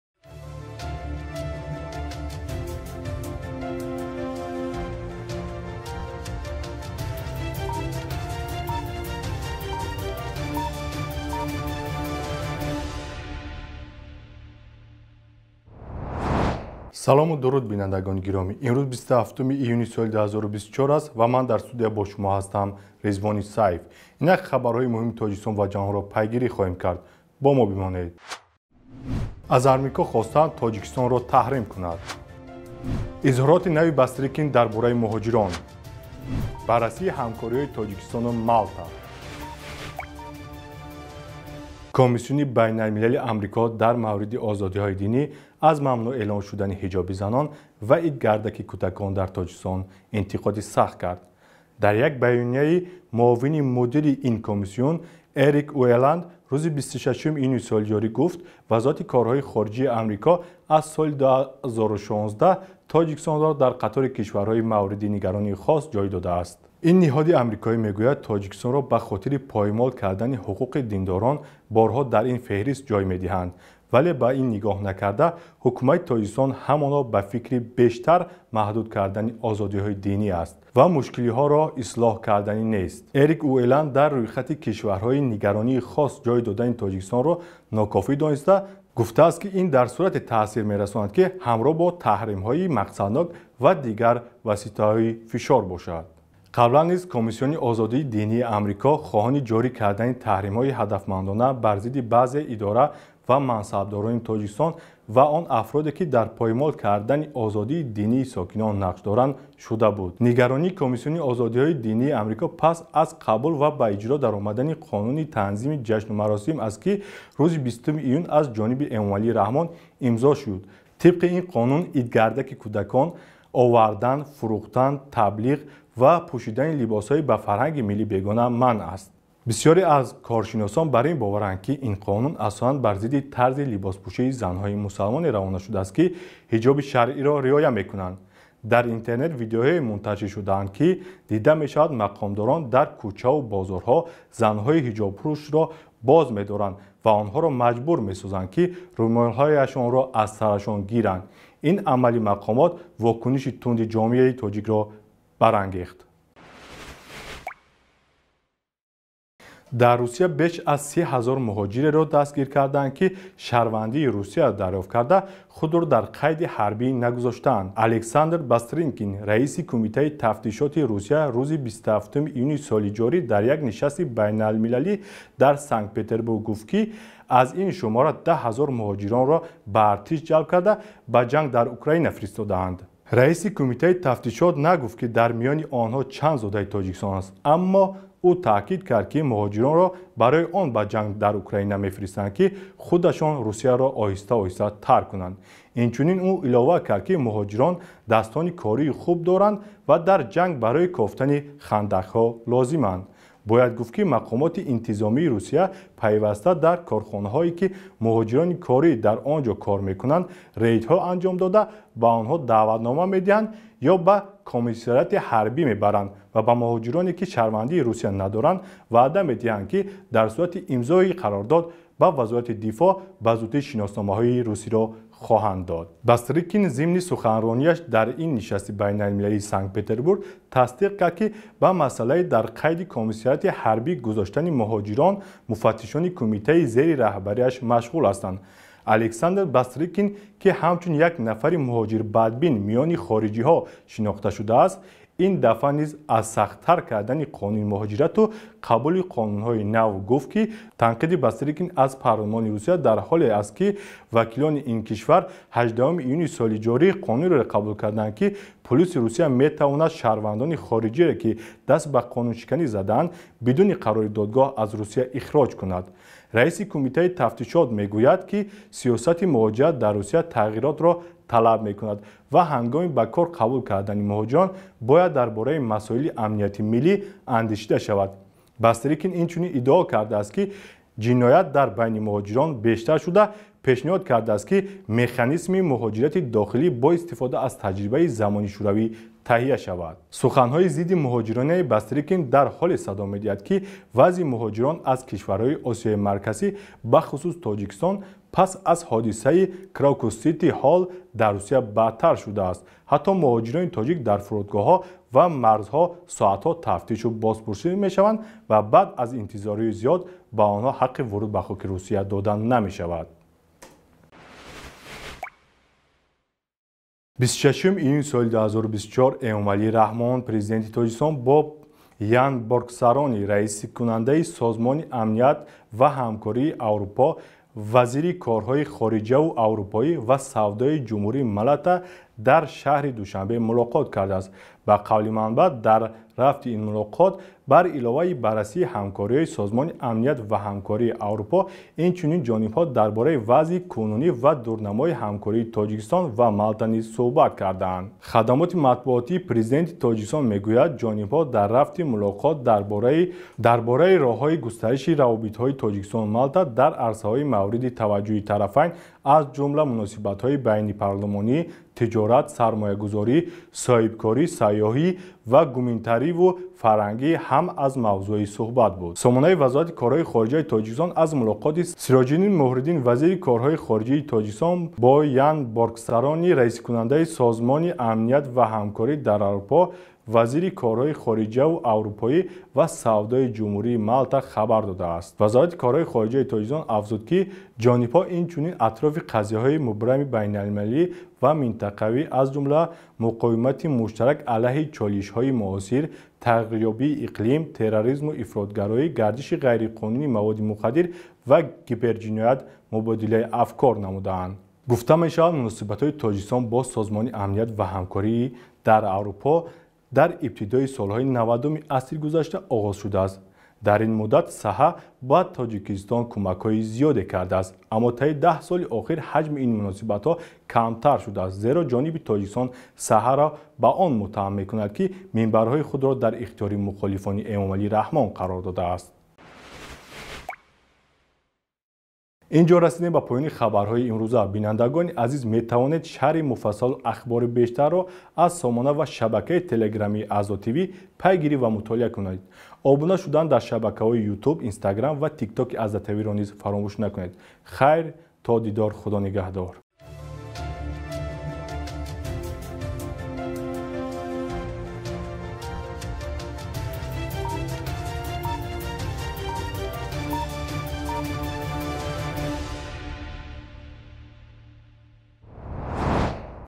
▶️Барномаи хaбарии ИМРӮЗ - 27.06.2024 | AZDА TV | برنامه ای خبری امروز اخبار تاجیکستان
Барномаи хaбарии ИМРӮЗ - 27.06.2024 | AZDА TV

-  Комиссюни байналмилалии Амрико дар мавриди озодиҳои динӣ (USCIRF) аз мамнуъ эълон шудани ҳиҷоби занон ва "идгардак"-и кӯдакон дар Тоҷикистон интиқоди сахт кард.

- Дар Русия беш аз 30 ҳазор муҳоҷиреро дастгир кардаанд, ки шаҳрвандии Русияро дарёфт карда, худро дар қайди ҳарбӣ нагузоштаанд.

-  26-уми июни соли 2024, Эмомалӣ Раҳмон, Президенти Тоҷикистон бо Ян Боргсарони, Раисикунандаи Созмони амният ва ҳамкорӣ дар Аврупо, Вазири корҳои хориҷиву аврупоӣ ва савдои Ҷумҳурии Малта дар шаҳри Душанбе мулоқот кардааст.